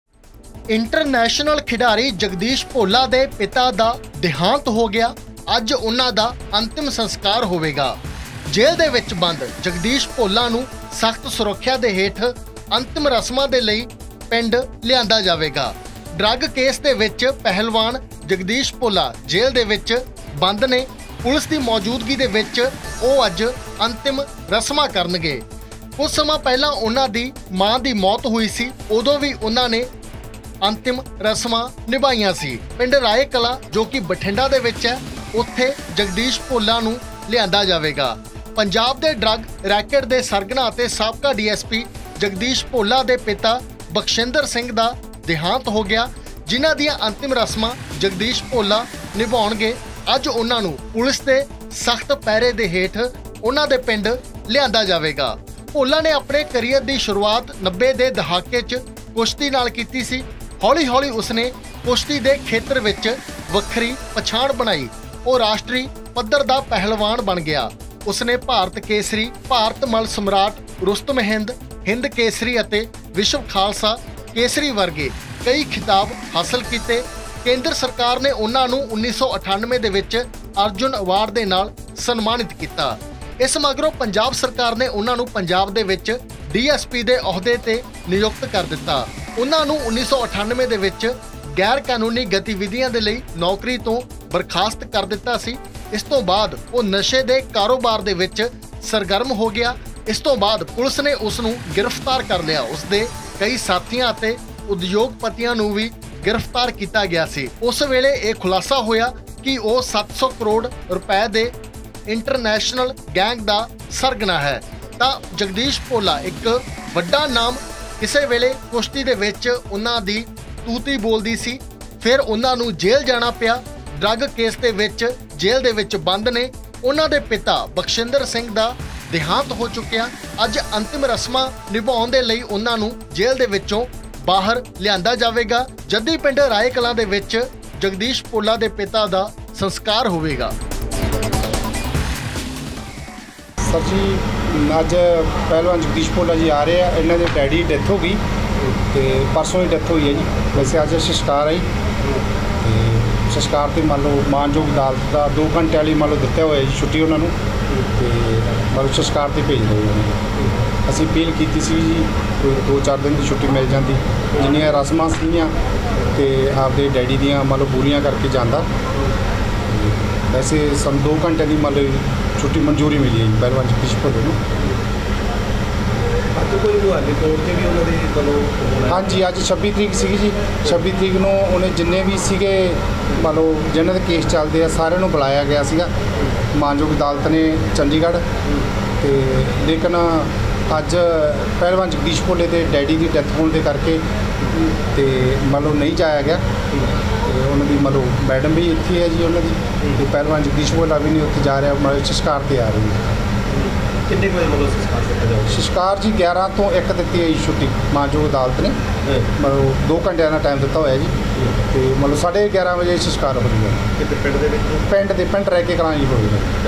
Malout News | ਜੇਲ੍ਹ 'ਚ ਬੰਦ Jagdish Bhola ਦੇ ਪਿਤਾ ਦਾ ਦੇਹਾਂਤ, ਅੰਤਿਮ ਰਸਮਾਂ ਨਿਭਾਉਣ ਲਈ ਆਉਣਗੇ ਬਾਹਰ | N18V
ਜੇਲ੍ਹ 'ਚ ਬੰਦ ਪਹਿਲਵਾਨ ਜਗਦੀਸ਼ ਭੋਲਾ ਦੇ ਪਿਤਾ ਦਾ ਦੇਹਾਂਤ, ਅੰਤਿਮ ਰਸਮਾਂ ਨਿਭਾਉਣ ਲਈ ਆਉਣਗੇ ਬਾਹਰ, ਨਸ਼ਾ ਤਸਕਰੀ ਦੇ ਮਾਮਲੇ 'ਚ ਕੱਟ ਰਹੇ ਨੇ ਸਜ਼ਾ